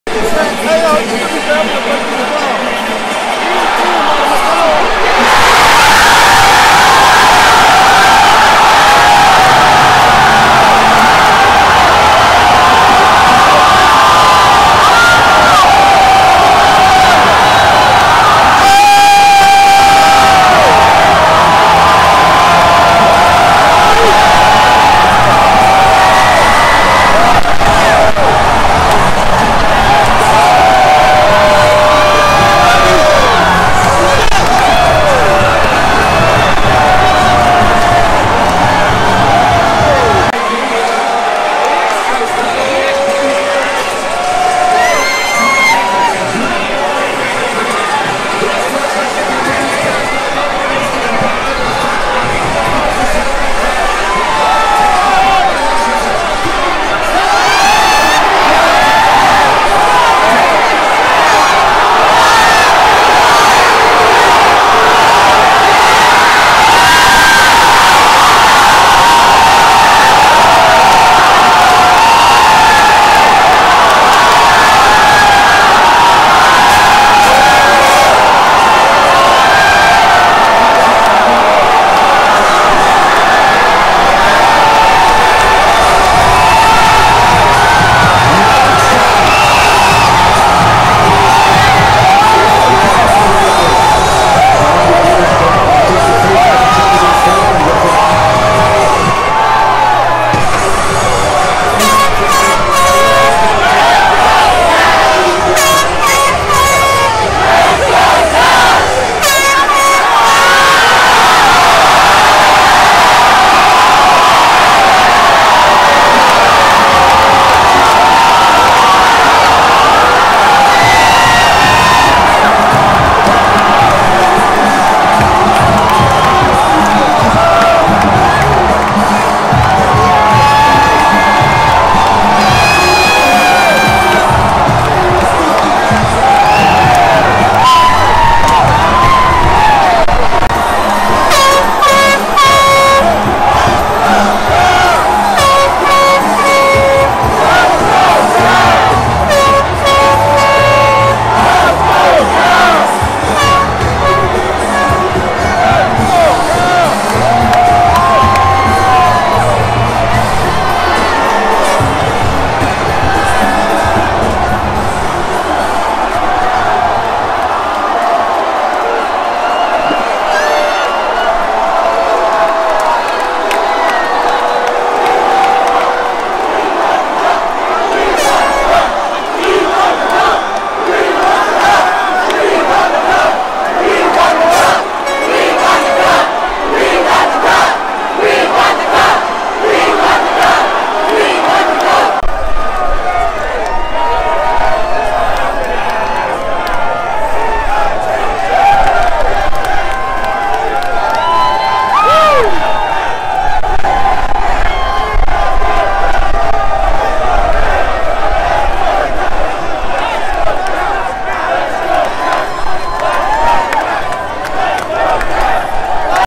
What it looked like in Washington as the Capitals won their first Stanley Cup | ESPN
Watch the fan reaction from D.C. as the Washington Capitals scored their Stanley Cup-winning goal, then held on to win their first title in history.